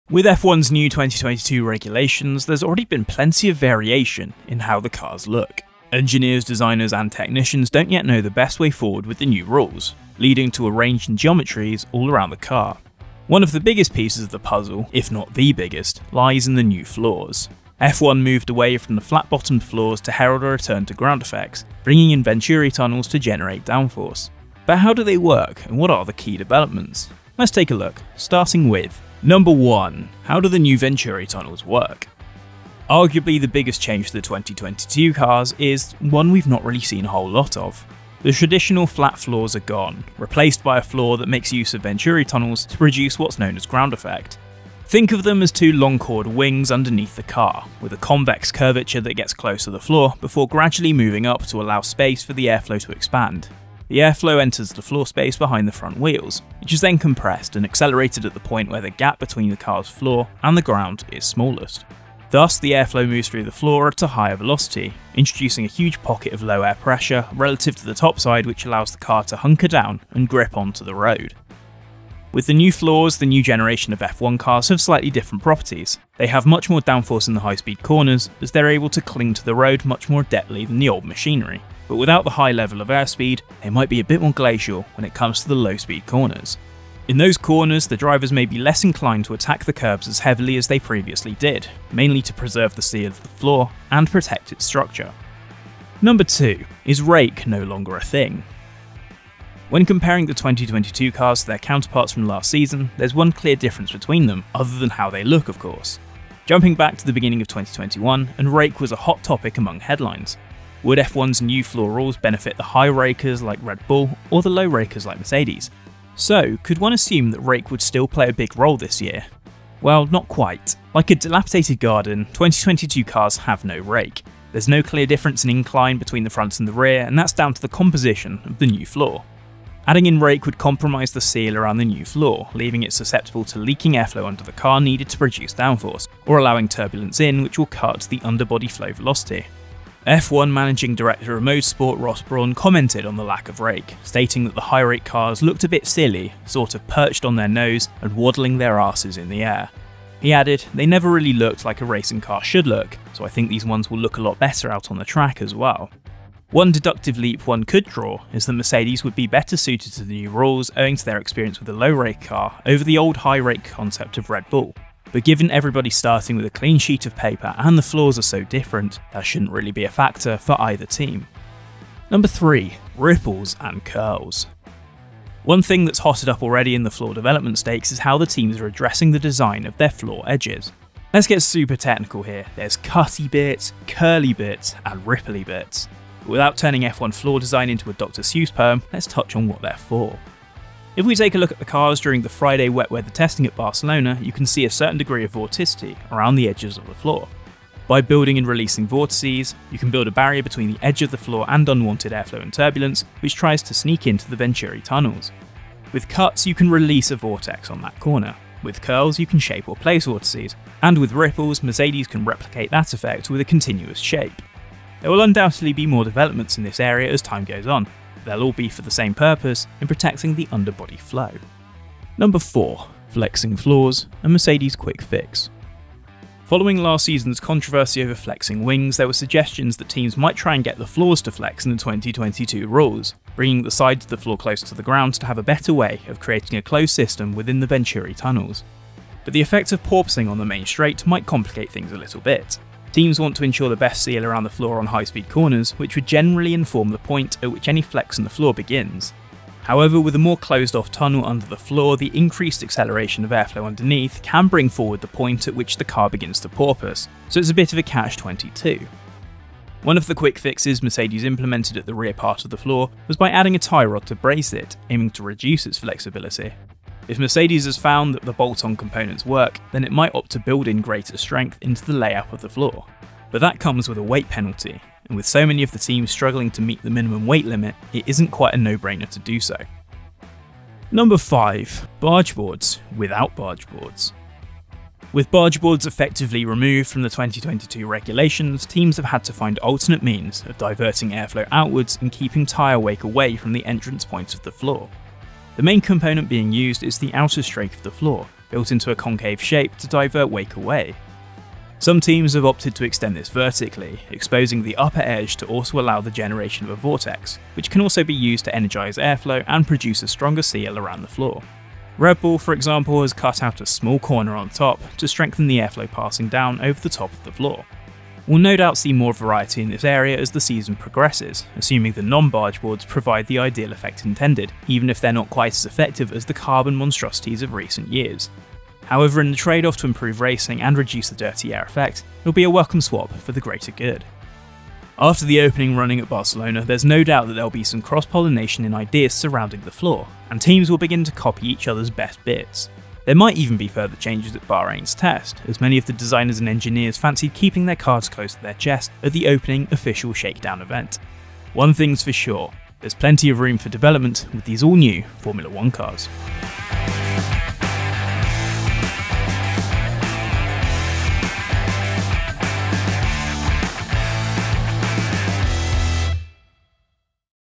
Why F1's Floors Could Hold The Key To Success In 2022
Do F1's floors hold the key to success under the 2022 Technical Regulations? Formula 1's overhaul may have impacted teams a lot, but there's been a lot of development getting the new Venturi Tunneled floors to maximise speeds, both in and out the corners. Jake Boxall-Legge takes a look at 5 developments Formula 1 teams have made to their floors during F1's Test.

0:00 F1's Floor Developments
0:29 Venturi Tunnels
1:35 Car Rake
2:52 Ripples and Curls
3:42 Flexi-Floors
4:40 Bargeboards without Bargeboards
5:18 Improving The Floors